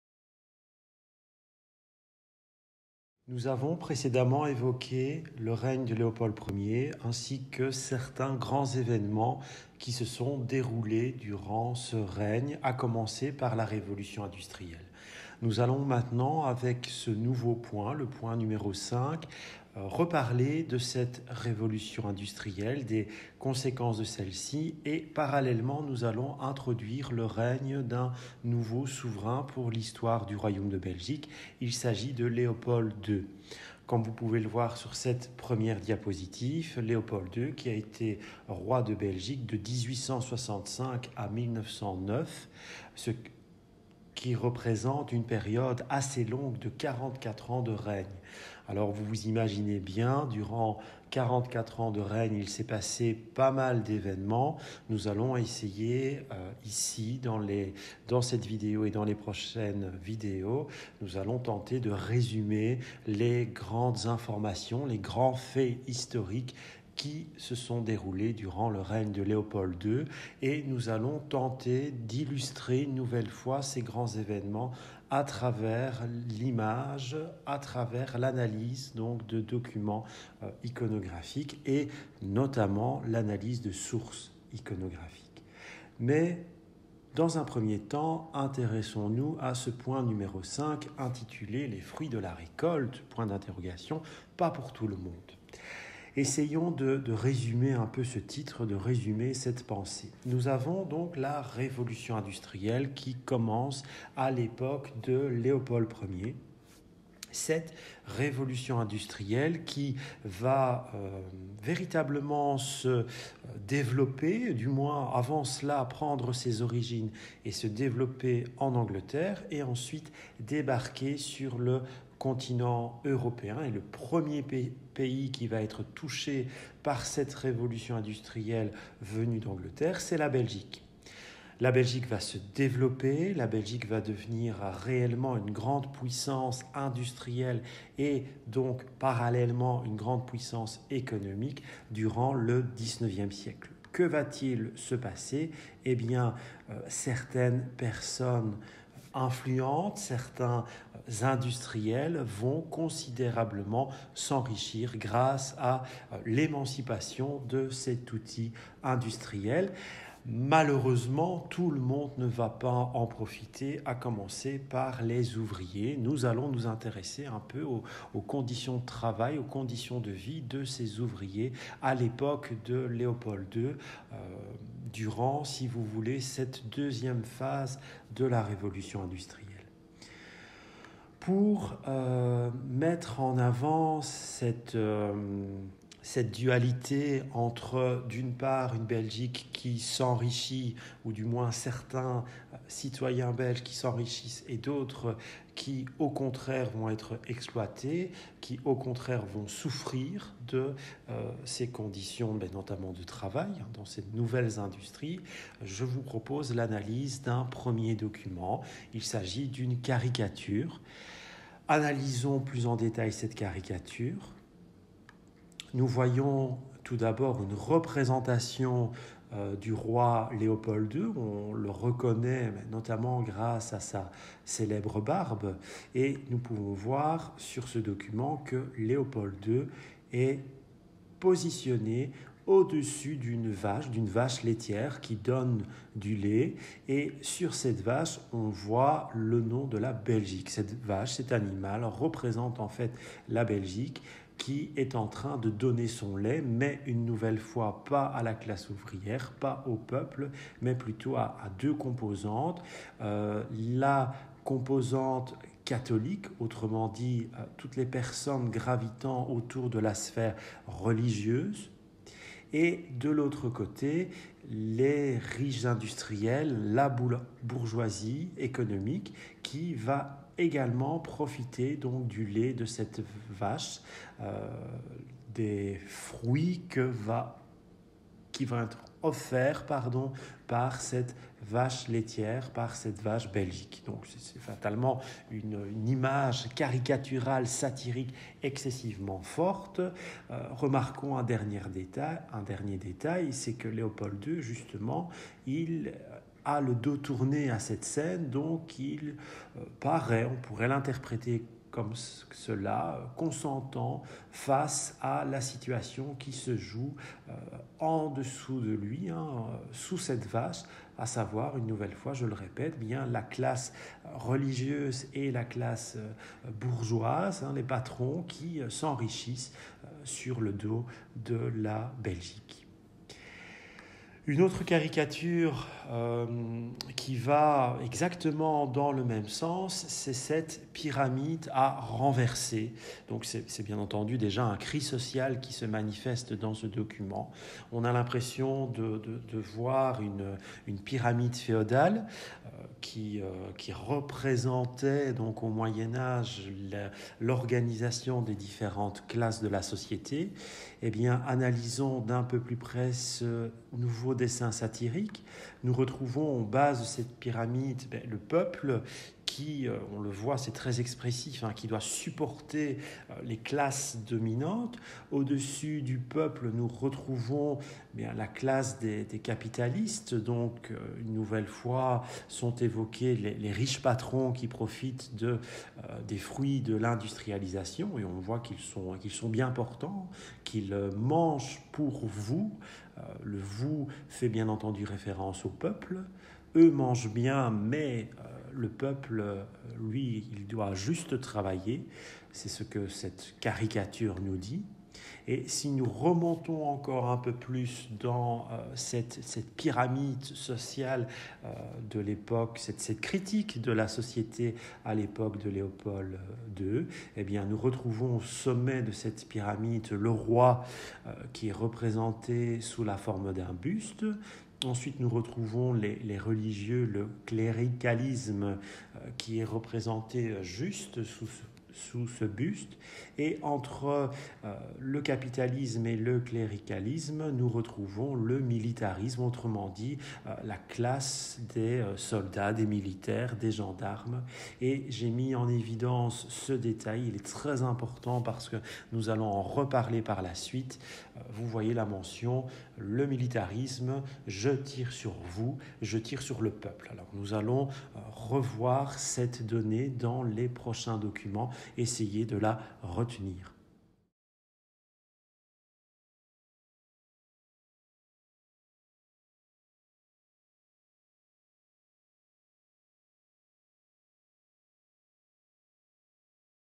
L'Histoire du Royaume de Belgique - Deuxième Partie - Vidéo n°6
Fiche n°5 - L'Histoire du Royaume de Belgique / Deuxième Partie : Léopold II, 44 ans de règne - Point 5 : Les fruits de la récolte ?